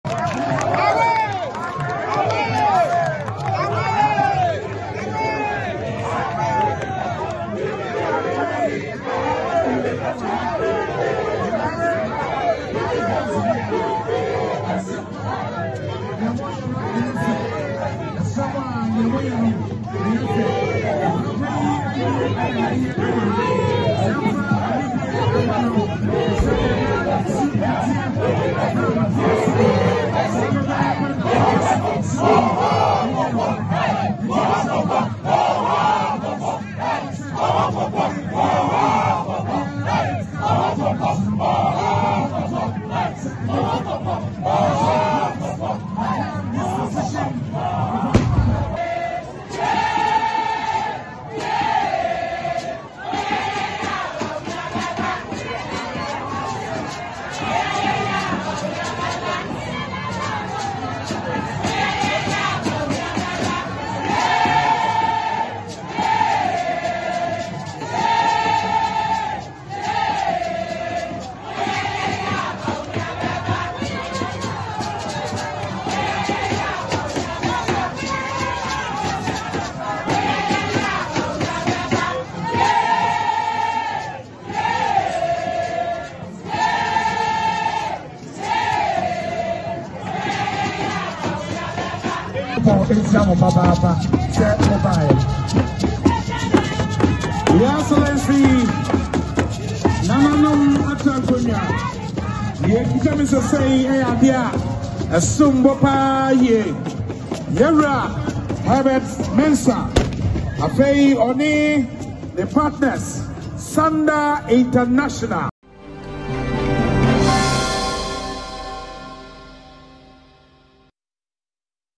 Bawumia, Alan supporters hoot at each other at Theresa Kufuor’s funeral
Supporters of the New Patriotic Party and followers of Alan Kyeremanten squared off during former First Lady Theresa Kufuor's final funeral rites.